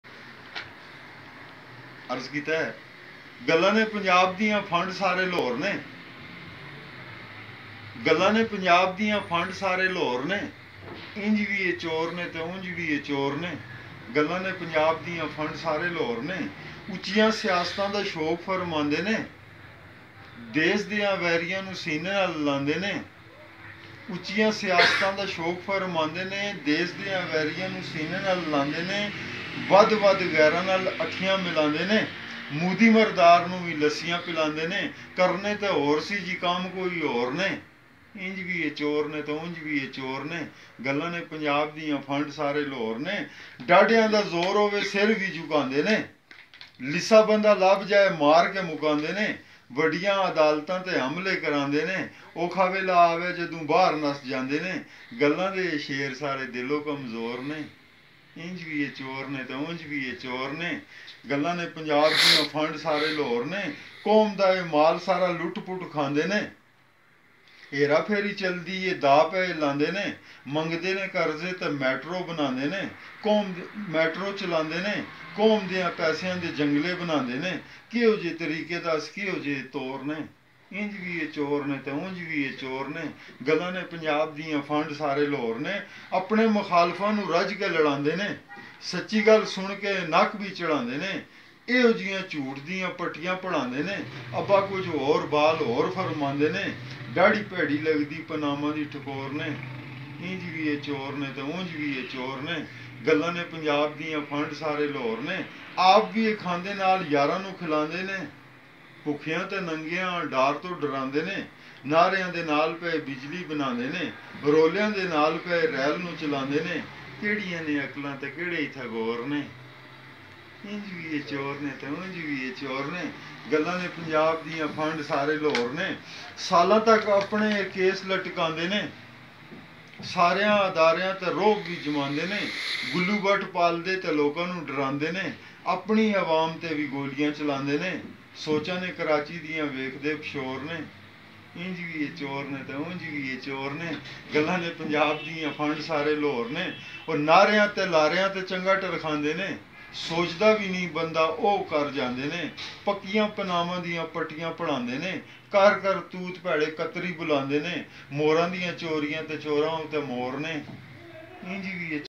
Panama Lines by Riaz Tahir
Best poet
Best Poetry about the rael picture of punjab and punjab government